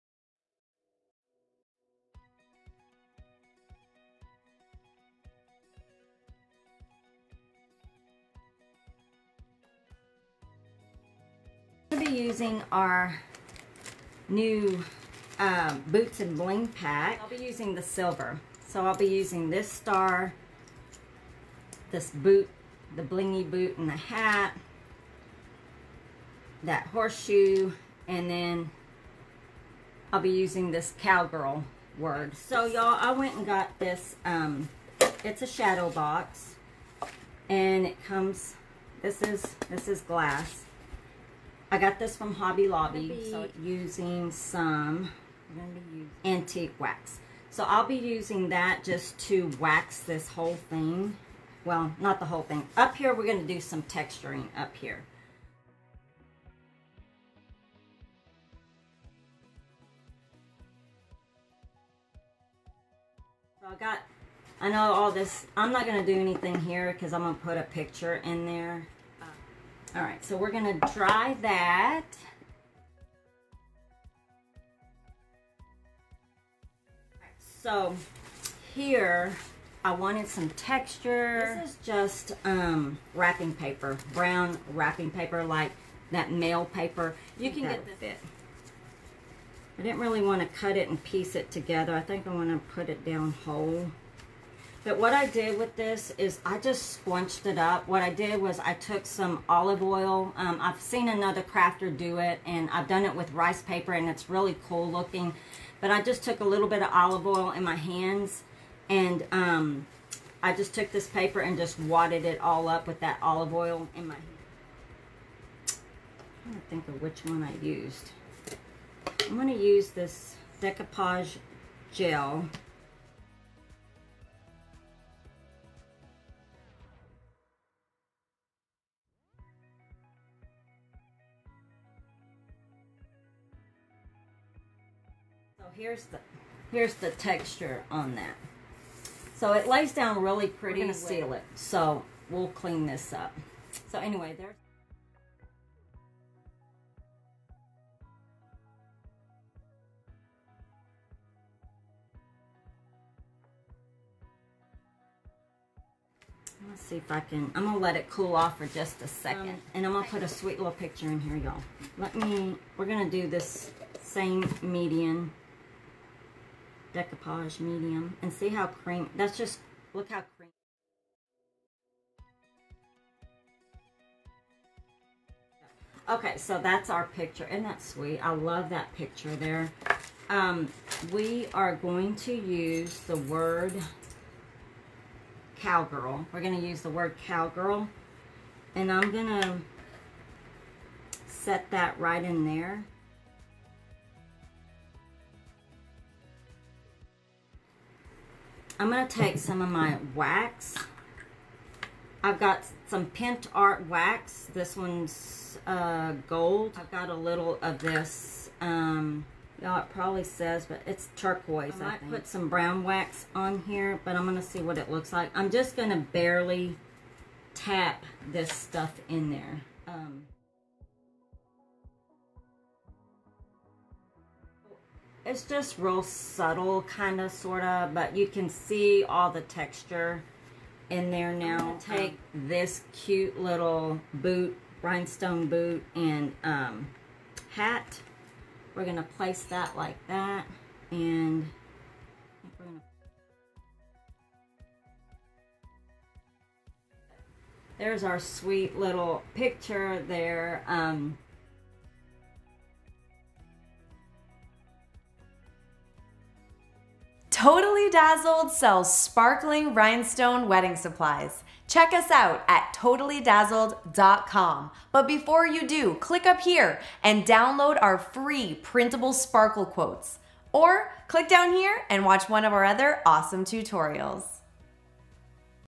Cowgirl Frame | Boots & Bling Frame
In this video tutorial from Totally Dazzled, Linda teaches you how to make a cowgirl frame using Totally Dazzled Boots and Bling pack. Find a complete list of supplies in the description below.

SUPPLIES:
Totally Dazzled Boots and Bling pack:

ABOUT TOTALLY DAZZLED
Totally Dazzled is a rhinestone craft supply company that helps sparkle lovers shine by providing education and tools to create glamorous crafts, weddings, events, and DIY’s.

Talent:
Linda | Circle Heart Creations